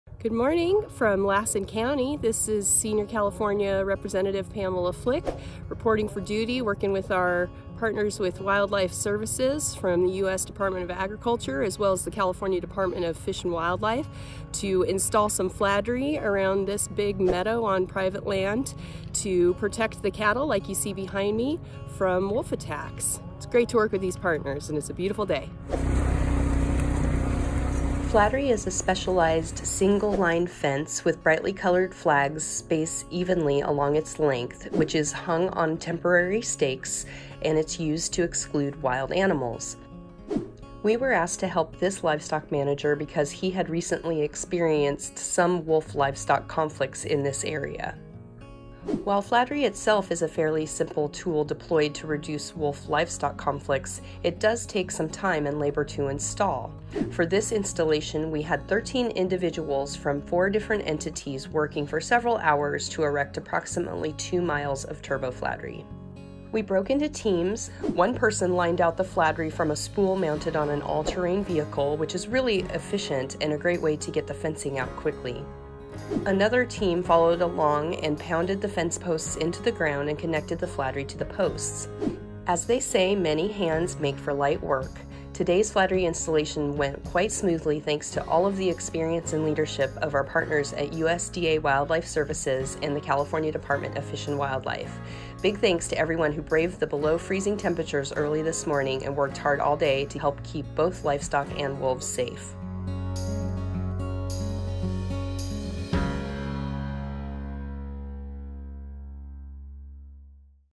Defenders: A Day in the Life - Reducing Wolf-Livestock Conflicts
Head to Lassen County California with Senior California Representative Pamela Flick where she works with a private landowner and agency partners to install over two miles of turbo fladry as a tool to help reduce wolf-livestock conflicts.

Video Transcript:
Good morning from Lassen County.

This is Senior California Representative Pamela Flick reporting for duty working with our partners with Wildlife Services from the US Department of Agriculture as well as the California Department of Fish and Wildlife

to install some fladry around this big meadow on private land to protect the cattle, like you see behind me, from wolf attacks.

It's great to work with these partners and it's a beautiful day.

(Machine installing fladry)

Fladry is a specialized single-line fence with brightly colored flags spaced evenly along its length, which is hung on temporary stakes and it's used to exclude wild animals.

We were asked to help this livestock manager because he had recently experienced some wolf-livestock conflicts in this area.

While fladry itself is a fairly simple tool deployed to reduce wolf-livestock conflicts it does take some time and labor to install.

For this installation, we had 13 individuals from 4 different entities working for several hours to erect approximately two miles of turbo fladry.

We broke into teams: one person lined out the fladry from a spool mounted on an all-terrain vehicle, which is really efficient and a great way to get the fencing out quickly.

Another team followed along and pounded the fence posts into the ground and connected the fladry to the posts.

As they say, many hands make for light work.

Today's fladry installation went quite smoothly thanks to all of the experience and leadership of our partners at USDA Wildlife Services and the California Department of Fish and Wildlife.

Big thanks to everyone who braved the below-freezing temperatures early this morning the below-freezing temperatures early this morning both livestock and wolves safe.